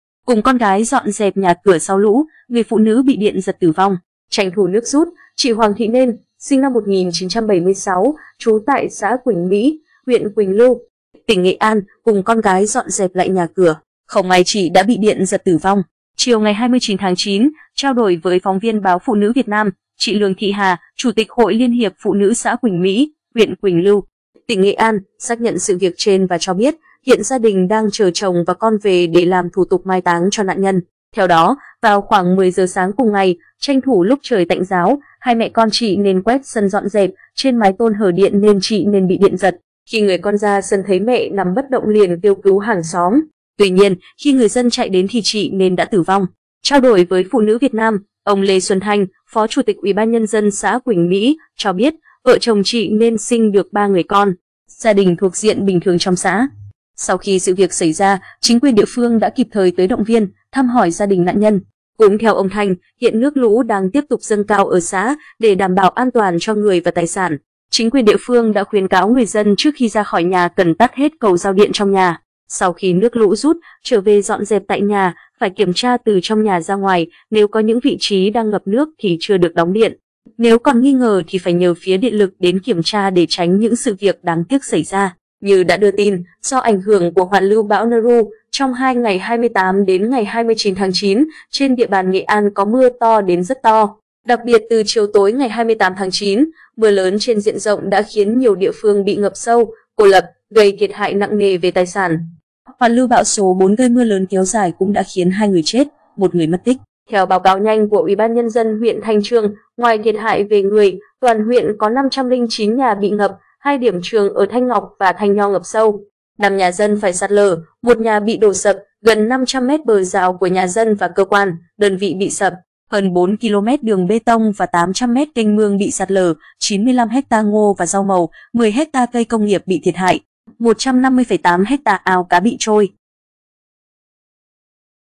Cùng con gái dọn dẹp nhà cửa sau lũ, người phụ nữ bị điện giật tử vong
Tranh thủ nước rút, chị Hoàng Thị Nên (SN 1976, trú tại xã Quỳnh Mỹ, huyện Quỳnh Lưu, tỉnh Nghệ An) cùng con gái dọn dẹp lại nhà cửa. Không may chị đã bị điện giật tử vong.Mưa lớn kéo dài khiến nhiều địa phương của huyện Thanh Chương bị chìm sâu trong nước
 Tính đến chiều ngày 29/9, mưa lũ ở Nghệ An đã khiến 2 người chết, 1 người mất tích
Chiều 29/9, trao đổi với PV Báo PNVN, chị Lương Thị Hà - Chủ tịch Hội LHPN xã Quỳnh Mỹ (huyện Quỳnh Lưu, tỉnh Nghệ An) - xác nhận sự việc trên và cho biết, hiện gia đình đang chờ chồng và con về để làm thủ tục mai táng cho nạn nhân.
Trao đổi với PNVN, ông Lê Xuân Thanh - Phó Chủ tịch UBND xã Quỳnh Mỹ - cho biết: "Vợ chồng chị Nên sinh được 3 người con, gia đình thuộc diện bình thường trong xã. Sau khi sự việc xảy ra, chính quyền địa phương đã kịp thời tới động viên, thăm hỏi gia đình nạn nhân".
Theo đó, vào khoảng 10h sáng cùng ngày, tranh thủ lúc trời tạnh ráo, hai mẹ con chị Nên quét sân dọn dẹp, trên mái tôn hở điện nên chị Nên bị điện giật. Khi người con ra sân thấy mẹ nằm bất động liền kêu cứu hàng xóm. Tuy nhiên, khi người dân chạy đến thì chị Nên đã tử vong.
Cũng theo ông Thanh, hiện nước lũ đang tiếp tục dâng cao ở xã, để đảm bảo an toàn cho người và tài sản, chính quyền địa phương đã khuyến cáo người dân trước khi ra khỏi nhà cần tắt hết cầu giao điện trong nhà. Sau khi nước lũ rút, trở về dọn dẹp tại nhà, phải kiểm tra từ trong nhà ra ngoài, nếu có những vị trí đang ngập nước thì chưa được đóng điện, nếu còn nghi ngờ thì phải nhờ phía điện lực đến kiểm tra để tránh những sự việc đáng tiếc xảy ra.
Như đã đưa tin, do ảnh hưởng của hoàn lưu bão Noru, trong 2 ngày 28-29/9, trên địa bàn Nghệ An có mưa to đến rất to. Đặc biệt từ chiều tối 28/9, mưa lớn trên diện rộng đã khiến nhiều địa phương bị ngập sâu, cô lập, gây thiệt hại nặng nề về tài sản. Hoàn lưu bão số 4 gây mưa lớn kéo dài cũng đã khiến 2 người chết, 1 người mất tích.
Theo báo cáo nhanh của UBND huyện Thanh Chương, ngoài thiệt hại về người, toàn huyện có 509 nhà bị ngập, 2 điểm trường ở Thanh Ngọc và Thanh Nho ngập sâu, 5 nhà dân phải sạt lở, 1 nhà bị đổ sập, gần 500 mét bờ rào của nhà dân và cơ quan, đơn vị bị sập, hơn 4km đường bê tông và 800 mét kênh mương bị sạt lở; 95 ha ngô và rau màu, 10ha cây công nghiệp bị thiệt hại, 150,8ha ao cá bị trôi...